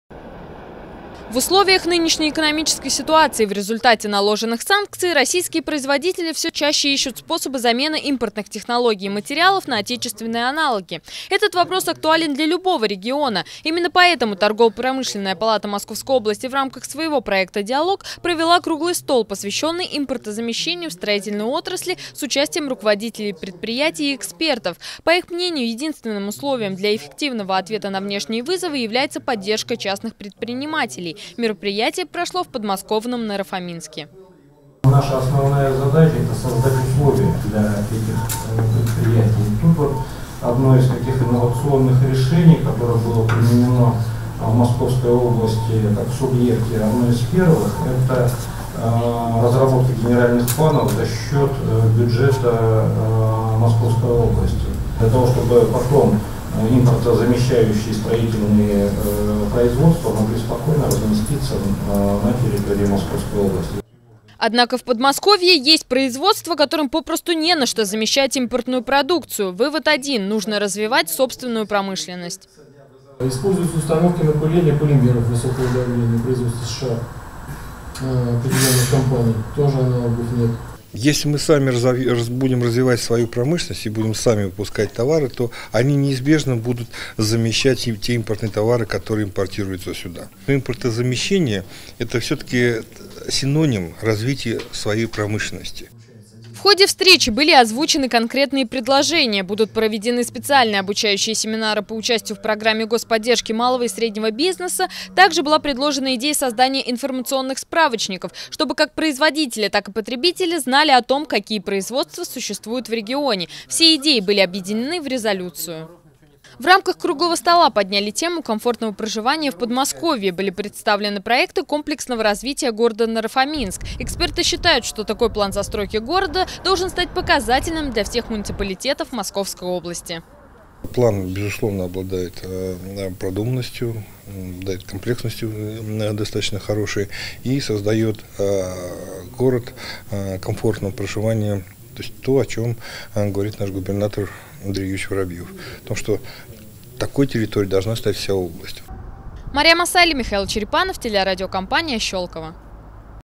Импортозамещение в строительной отрасли
В Подмосковном Наро-фоминске торгово-промышленная палата Московской области  в рамках своего проекта «Диалог» провела круглый стол, посвященный импортозамещению в строительной отрасли. В совещании участвовали руководители предприятий и эксперты